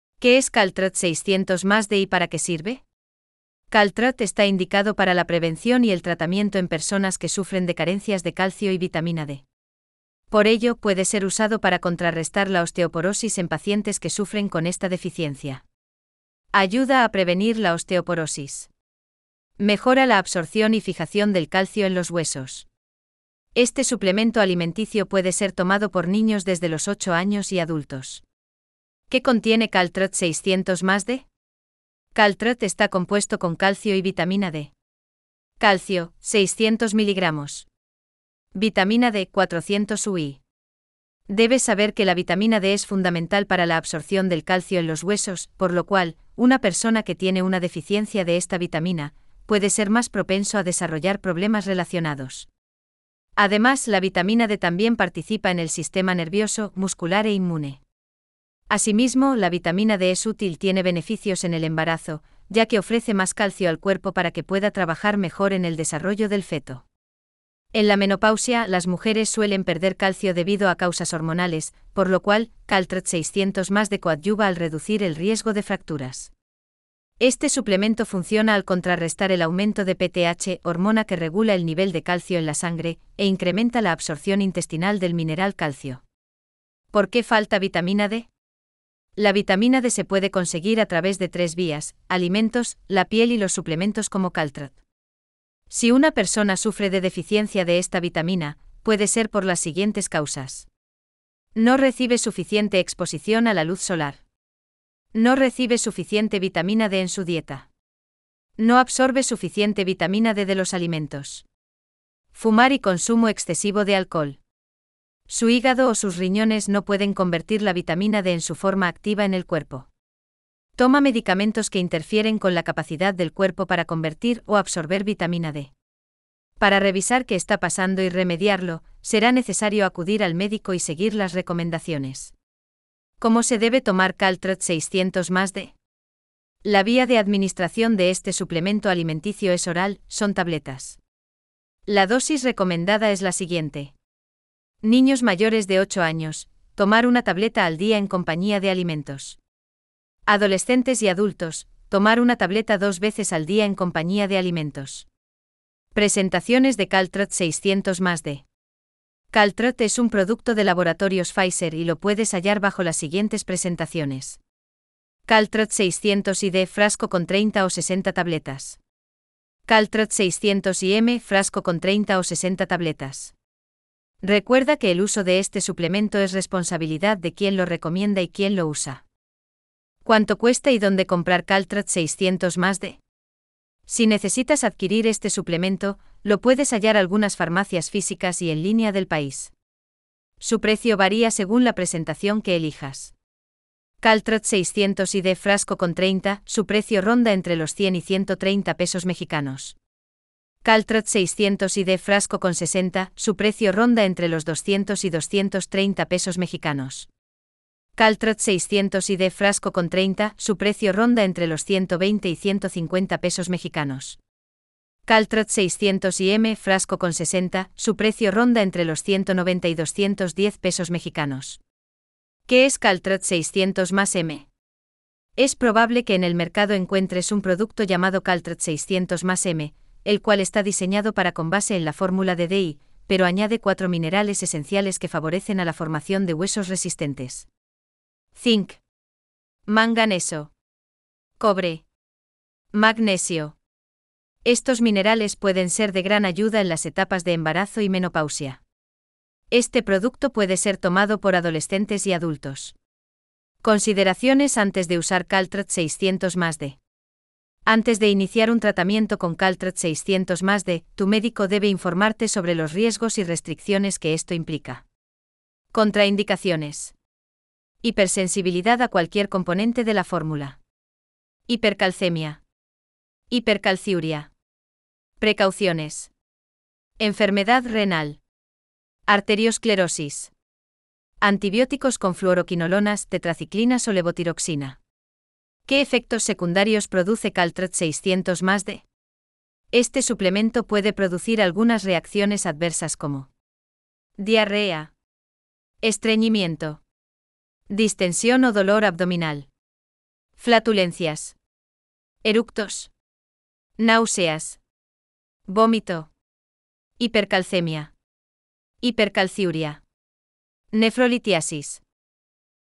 CALTRATE 600+D review, caltrate 600+d PARA QUE SIRVE, caltrate 600+d BENEFICIOS
¿Qué es Caltrate 600+D y para qué sirve?

Caltrate está indicado para la prevención y el tratamiento en personas que sufren de carencias de calcio y vitamina D.
Por ello,  puede ser usado para contrarrestar la osteoporosis en pacientes que sufren con esta deficiencia.

    Ayuda a prevenir la osteoporosis.
    Mejora la absorción y fijación del calcio en los huesos.

Este suplemento alimenticio puede ser tomado por niños desde los 8 años y adultos.

¿Qué contiene Caltrate 600+D?

Caltrate está compuesto con calcio y vitamina D.

    Calcio: 600 mg.
    Vitamina D: 400UI.

Debes saber que la vitamina D es fundamental para la absorción del calcio en los huesos,  por lo cual,  una persona que tiene una deficiencia de esta vitamina,  puede ser más propenso a desarrollar problemas relacionados.  Además,  la vitamina D también participa en el sistema nervioso,  muscular e inmune.

Asimismo,  la vitamina D es útil tiene beneficios en el embarazo,  ya que ofrece más calcio al cuerpo para que pueda trabajar mejor en el desarrollo del feto.

En la menopausia,  las mujeres suelen perder calcio debido a causas hormonales,  por lo cual,  Caltrate 600+D coadyuva al reducir el riesgo de fracturas.

Este suplemento funciona al contrarrestar el aumento de PTH (hormona que regula el nivel de calcio en la sangre) e incrementa la absorción intestinal del mineral calcio.

¿Por qué falta vitamina D?

La vitamina D se puede conseguir a través de tres vías: alimentos,  la piel y los suplementos como Caltrate.
Si una persona sufre de deficiencia de esta vitamina,  puede ser por las siguientes causas.

    No recibe suficiente exposición a la luz solar.
    No recibe suficiente vitamina D en su dieta.
    No absorbe suficiente vitamina D de los alimentos.
    Fumar y consumo excesivo de alcohol.
    Su hígado o sus riñones no pueden convertir la vitamina D en su forma activa en el cuerpo.
    Toma medicamentos que interfieren con la capacidad del cuerpo para convertir o absorber vitamina D.

Para revisar que está pasando y remediarlo,  será necesario acudir al médico y seguir las recomendaciones.

¿Cómo se debe tomar Caltrate 600+D?

La vía de administración de este suplemento alimenticio es oral,  son tabletas.

La dosis recomendada es la siguiente:

    Niños mayores de 8 años: Tomar 1 tableta al día en compañía de alimentos.

    Adolescentes y adultos: Tomar 1 tableta 2 veces al día en compañía de alimentos.

Presentaciones de Caltrate 600+D.

Caltrate es un producto de laboratorios Pfizer y lo puedes hallar bajo las siguientes presentaciones:

    Caltrate 600+D,  frasco con 30 o 60 tabletas.
    Caltrate 600+M,  frasco con 30 o 60 tabletas.

Recuerda que el uso de este suplemento es responsabilidad de quien lo recomienda y quien lo usa.

¿Cuánto cuesta y dónde comprar Caltrate 600+D?

Si necesitas adquirir este suplemento,  lo puedes hallar algunas farmacias físicas y en línea del país.  Su precio varía según la presentación que elijas.

    Caltrate 600+D,  frasco con 30, su precio ronda entre los 100 y 130 pesos mexicanos.

    Caltrate 600+D,  frasco con 60, su precio ronda entre los 200 y 230 pesos mexicanos.

    Caltrate 600+D,  frasco con 30, su precio ronda entre los 120 y 150 pesos mexicanos.

    Caltrate 600+M,  frasco con 60, su precio ronda entre los 190 y 210 pesos mexicanos.

¿Qué es Caltrate 600+M?

Es probable que en el mercado encuentres un producto llamado Caltrate 600+M,  el cual está diseñado para con base en la fórmula de D+,  pero añade 4 minerales esenciales que favorecen a la formación de huesos resistentes.

    Zinc.
    Manganeso.
    Cobre.
    Magnesio.

Estos minerales pueden ser de gran ayuda en las etapas de embarazo y menopausia.

Este producto puede ser tomado por adolescentes y adultos.

Consideraciones antes de usar Caltrate 600+D.

Antes de iniciar un tratamiento con Caltrate 600+D,  tu médico debe informarte sobre los riesgos y restricciones que esto implica.

Contraindicaciones.

    Hipersensibilidad a cualquier componente de la fórmula.
    Hipercalcemia.
    Hipercalciuria.

Precauciones.

    Enfermedad renal.
    Arteriosclerosis.
    Antibióticos con fluoroquinolonas,  tetraciclinas o levotiroxina.

¿Qué efectos secundarios produce Caltrate 600+D?

Este suplemento puede producir algunas reacciones adversas como:

    Diarrea.
    Estreñimiento.
    Distensión o dolor abdominal.
    Flatulencias.
    Eructos.
    Náuseas.
    Vómito.
    Hipercalcemia.
    Hipercalciuria.
    Nefrolitiasis.